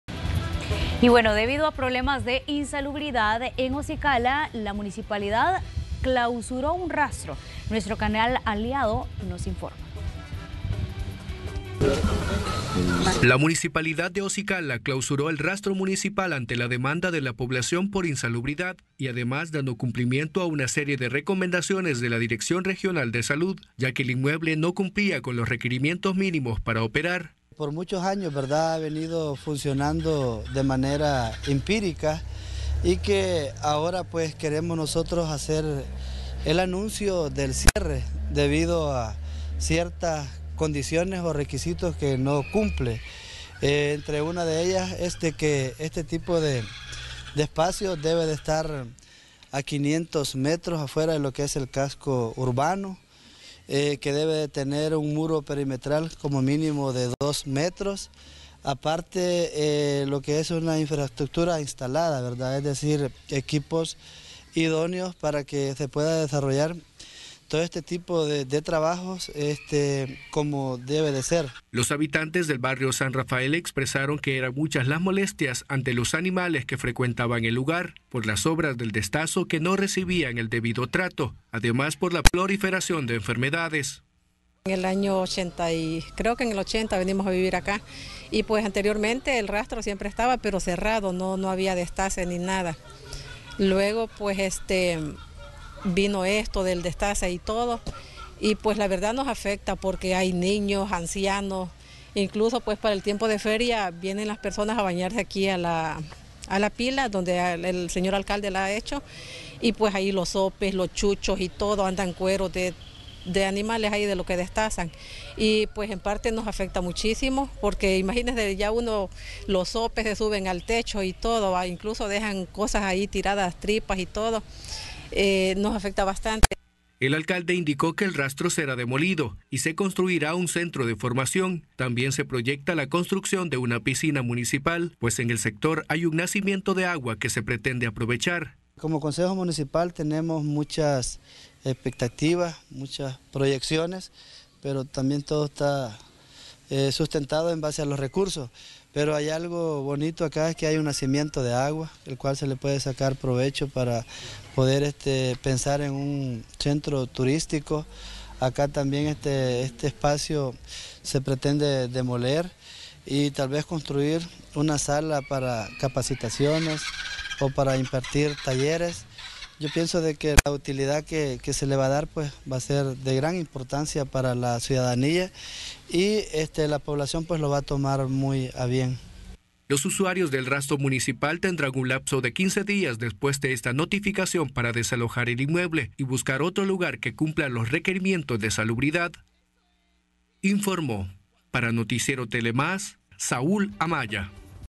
Clausuran rastro municipal en Osicala
El inmueble no cumplía con los requerimientos mínimos para operar afectando a los habitantes del barrio San Rafael.

La población asegura que sufrían muchas molestias entre ellas que los perros y sopes frecuentaban el lugar por las sobras del destazo que no recibían el debido trato, además de la proliferación de enfermedades que afectaba a las familias que habitan en la zona.